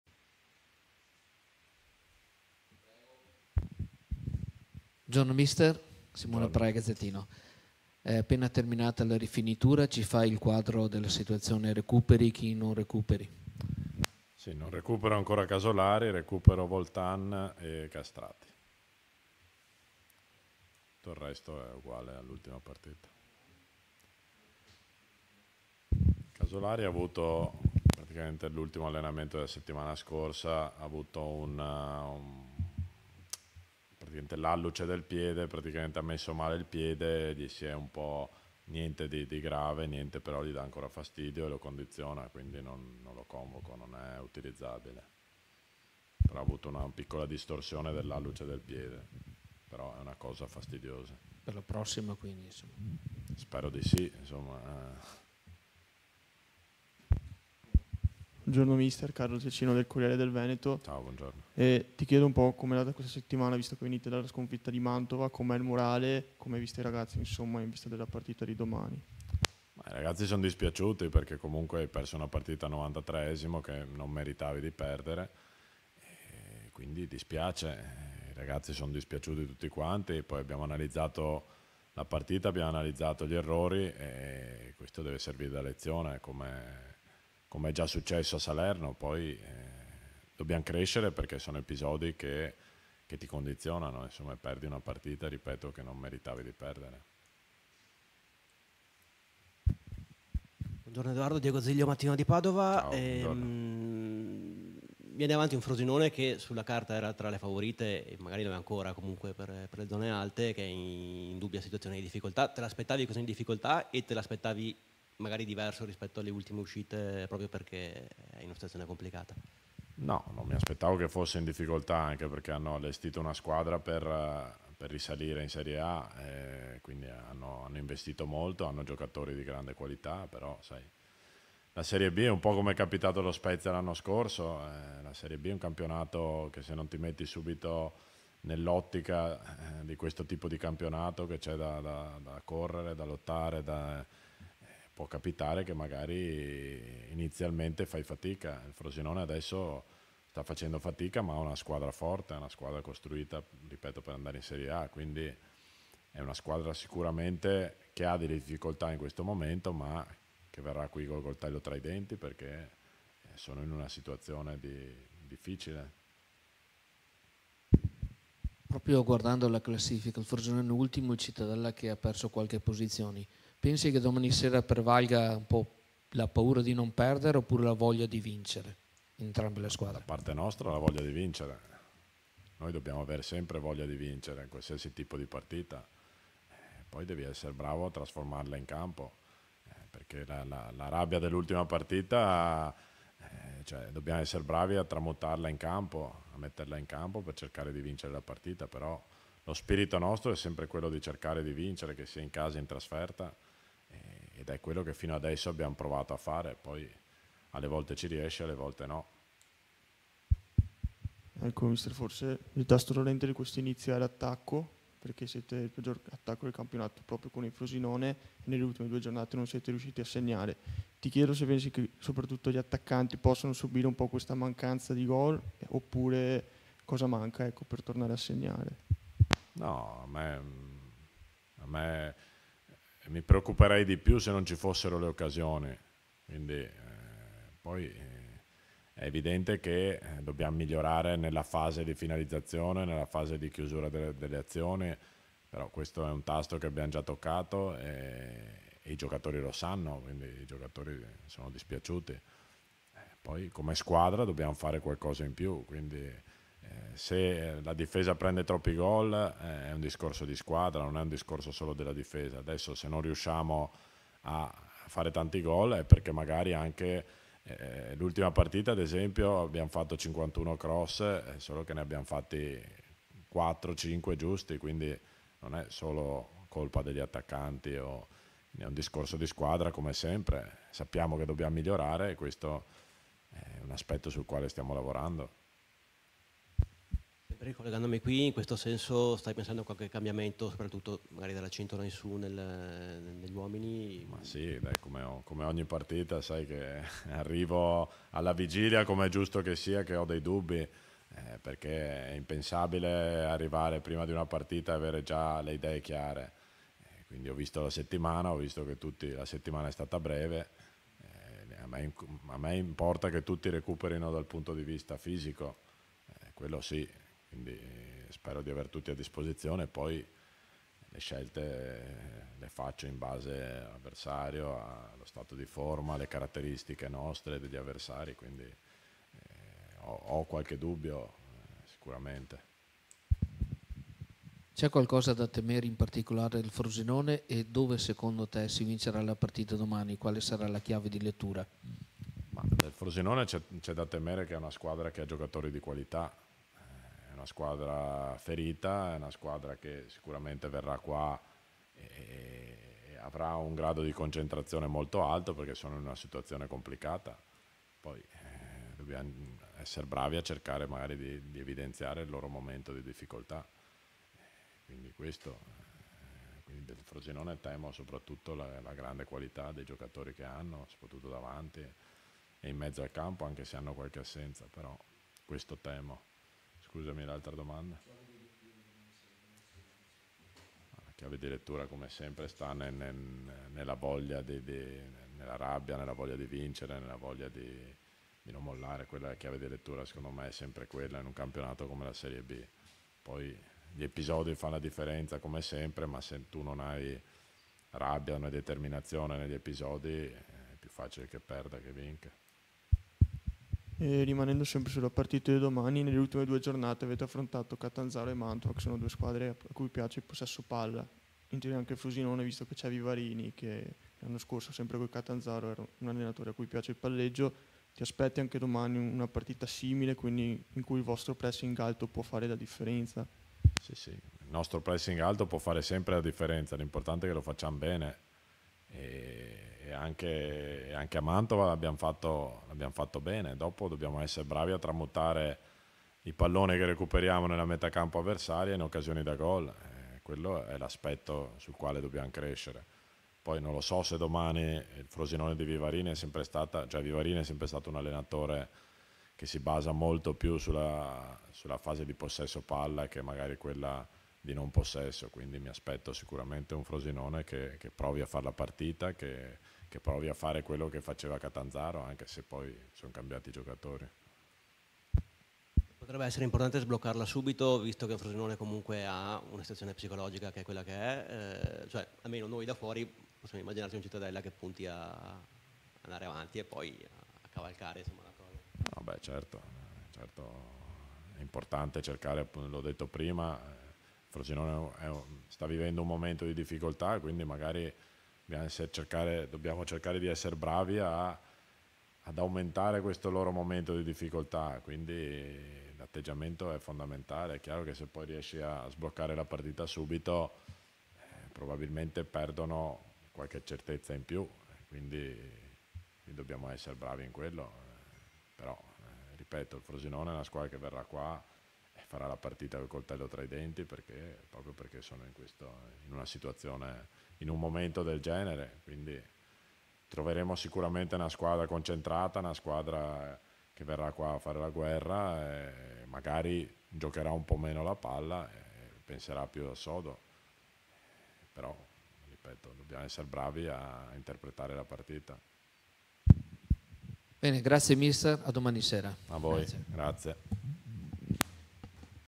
Cittadella - Frosinone | Press conference
Citta e Frosinone aprono la 7^ giornata di B.
Le parole di Mister Gorini dopo la rifinitura odierna.